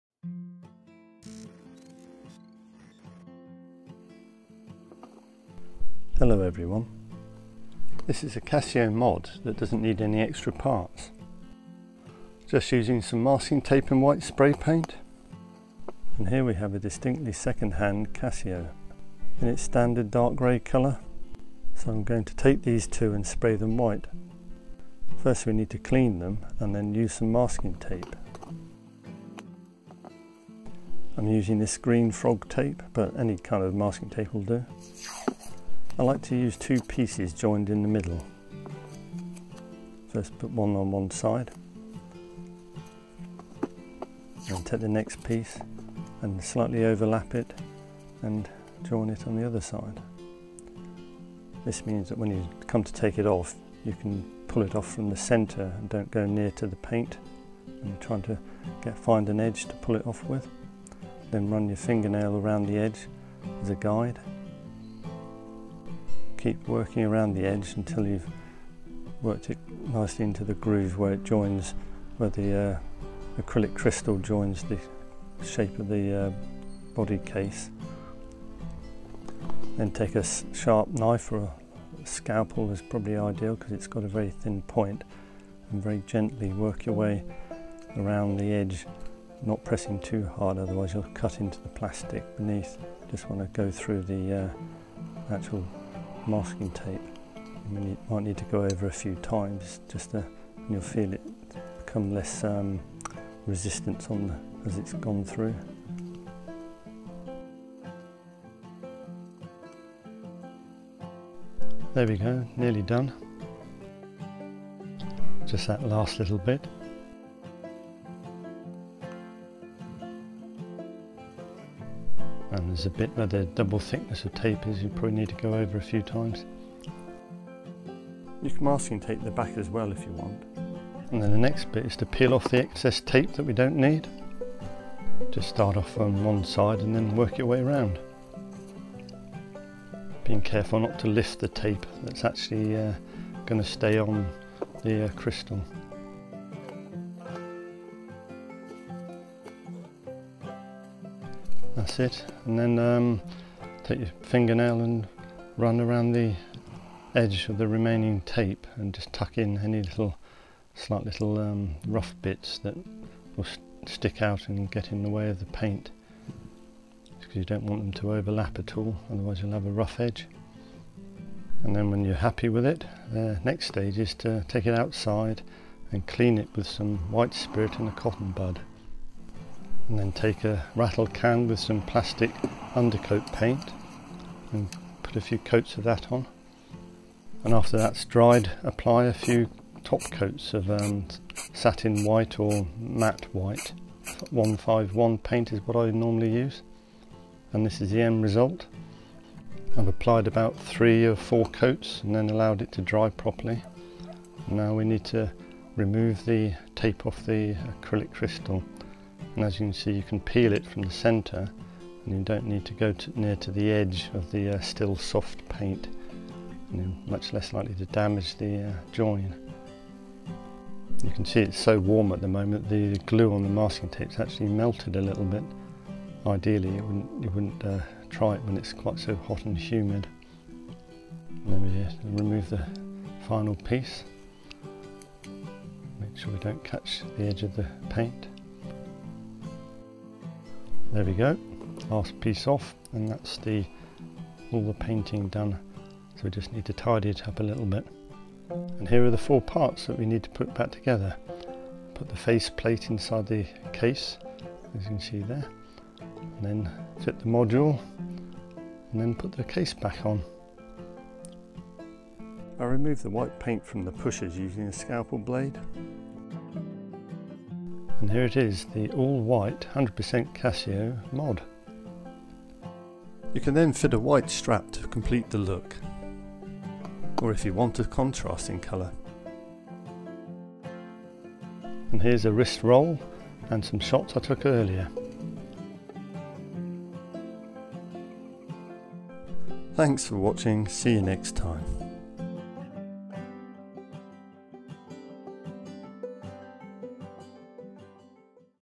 Casio Royale mod with masking tape and spray paint.
A quick video showing how I use masking tape and spray paint to modify my Casios 👍